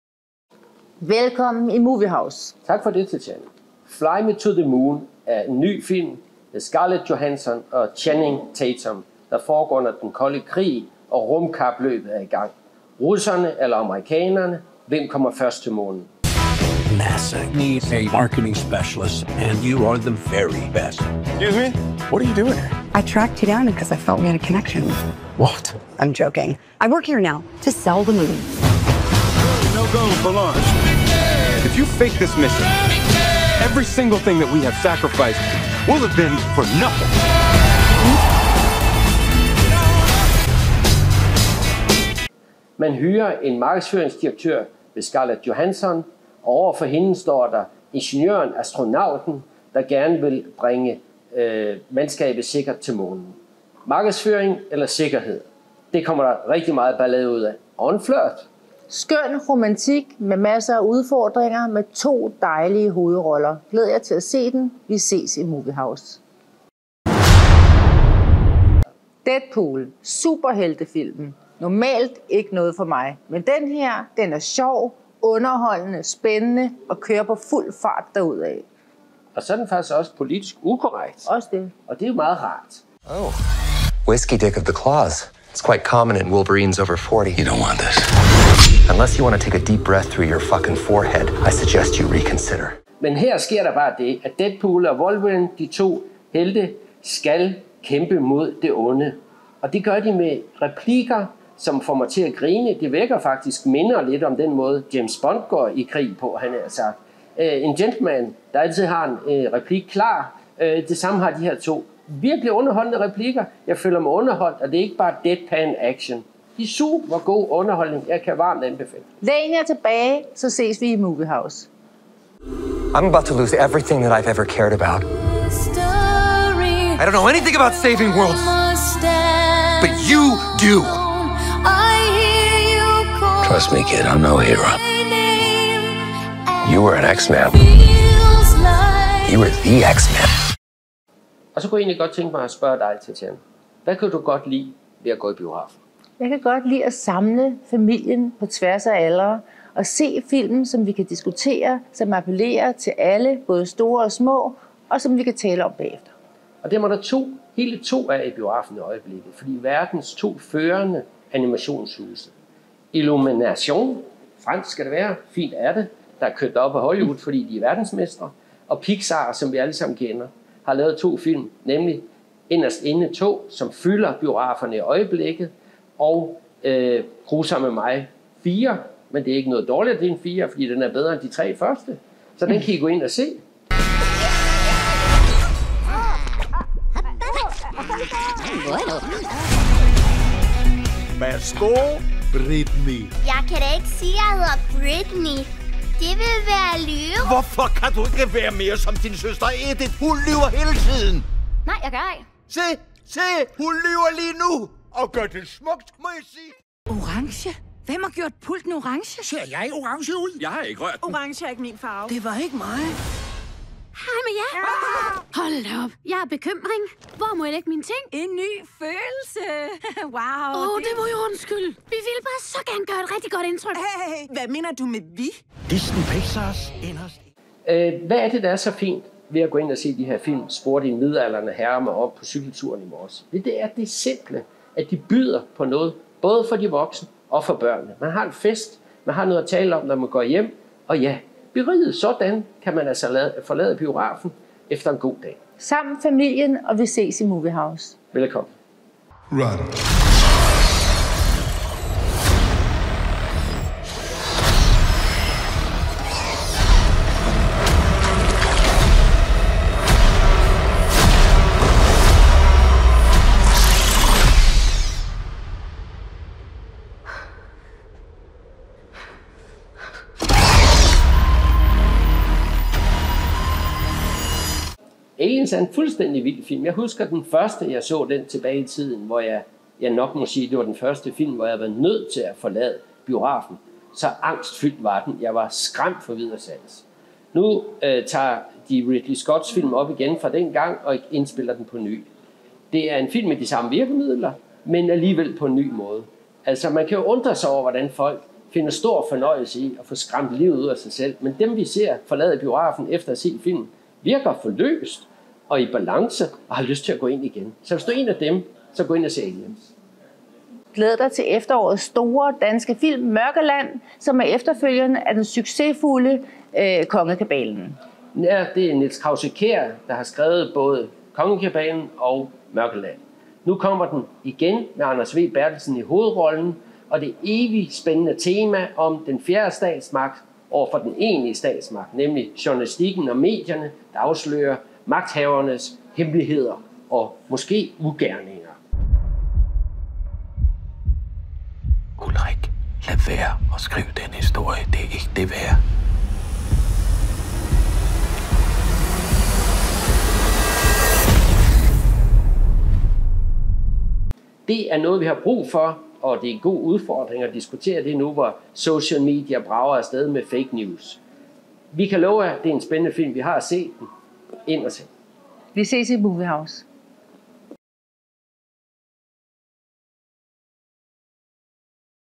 Sommer film i MovieHouse
Tatjana og Michael Obel har set sommerens store film. Hør hvorfor du skal se dem.